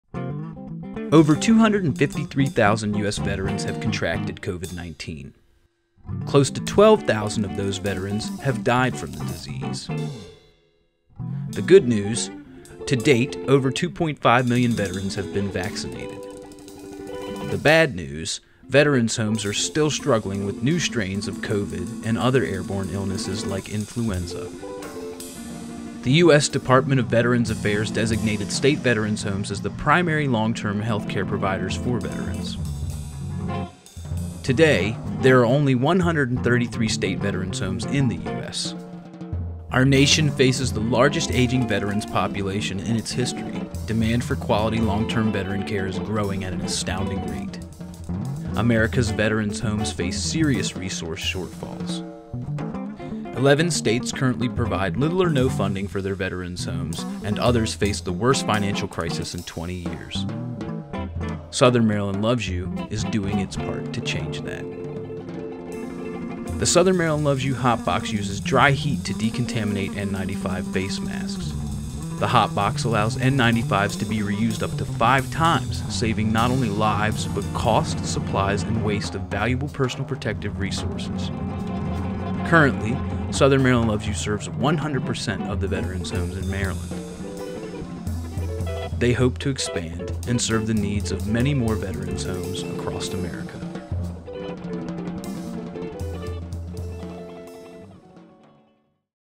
SOMDLY Supports Our Veterans
Over 253,000 U.S. Veterans have contracted COVID-19... Close to 12,000 of those have died from the disease.

If you know of a Veterans Home short on PPE, please have them reach out. SoMD Loves You wants to help now.

Film Credit: Dominic Fragman